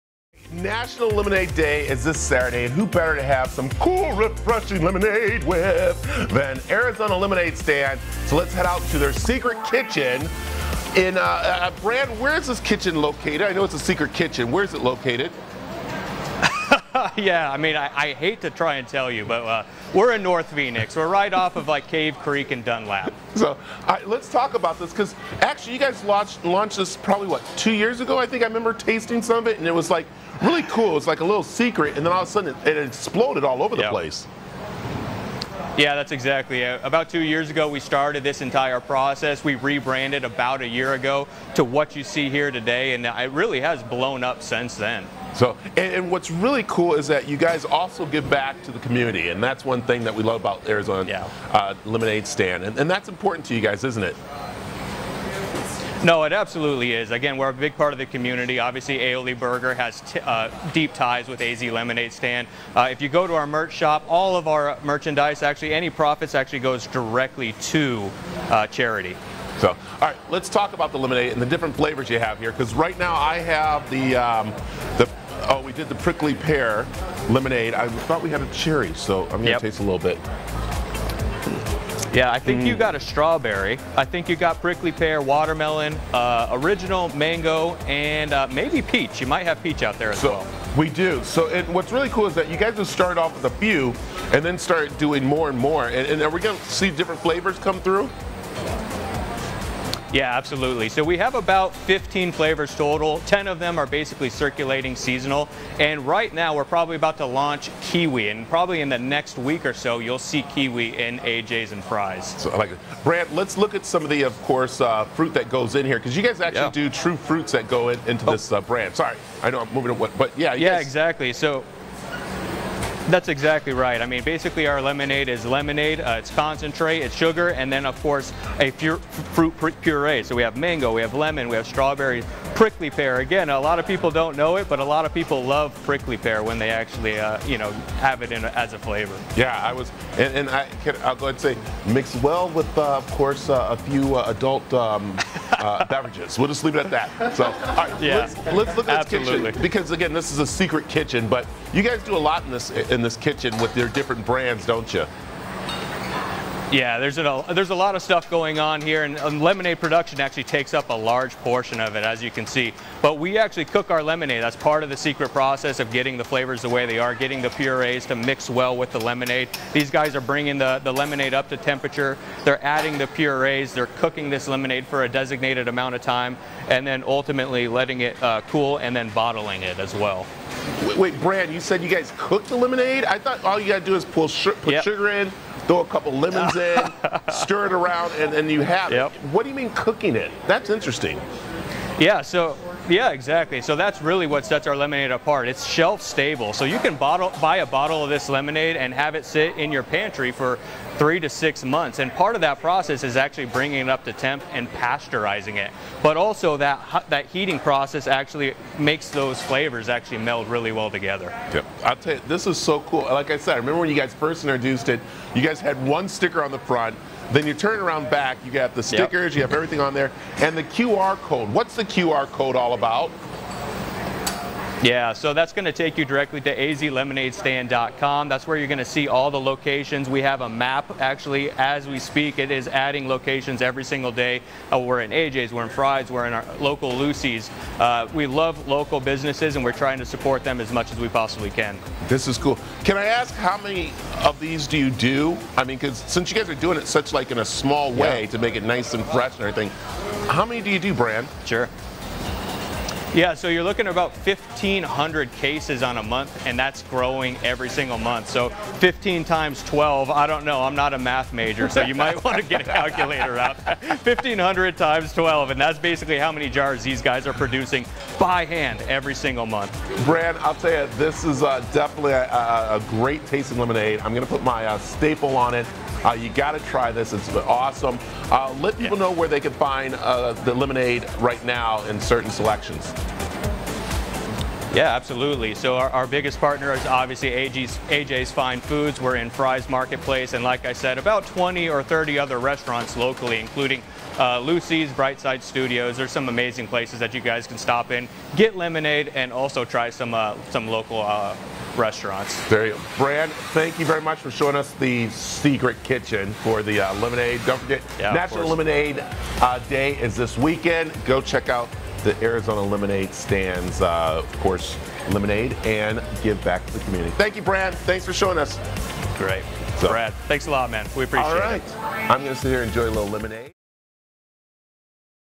Celebrate National Lemonade Day With AZ Lemonade Stand
National Lemonade Day is this coming Saturday! Let’s celebrate with a behind the scenes look at how your favorite local lemonade is made at AZ Lemonade Stand, with more than 15 fresh flavors available like watermelon, prickly pear, peach, mango, and strawberry. Available at all Aioli Burger locations, Fry’s, Luci's Market, AJ's Fine Foods, My Pie Pizza, Lookout Tavern and more. Visit online for more info and recipes at azlemonadestand.com.